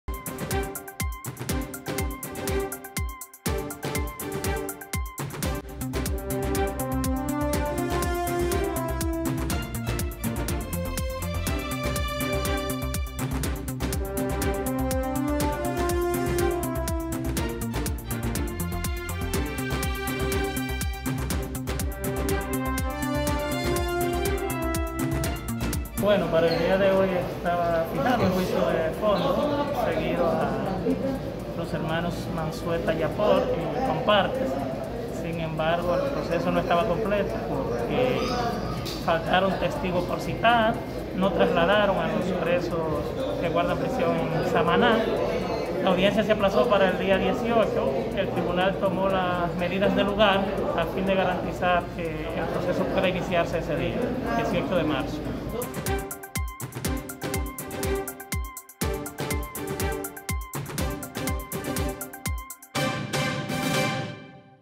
Reenvían juicio de fondo seguidos a acusado de asesinar a pareja en Arenoso
SAN FRANCISCO DE MACORIS. - El Juzgado de la Instrucción del distrito judicial de Duarte, reenvió para el próximo 18 de marzo el inicio del conocimiento del juicio de fondo contra los hermanos Abel Manzueta Yapor y Ecno Miguel Manzueta Yapor, acusado del asesinato de una pareja de esposo en el municipio de Arenoso.

El abogado Francisco Rodríguez, expresó que la audiencia fue aplazada debido a que no se trasladaron los enjuiciados y faltar testigos por citar.

A los hermanos se les acusa de haber participado en la muerte de la pareja de esposos Wolter Fernando Peña Almánzar y Florindy J. Lajara, en el municipio de Arenoso en diciembre de 2018.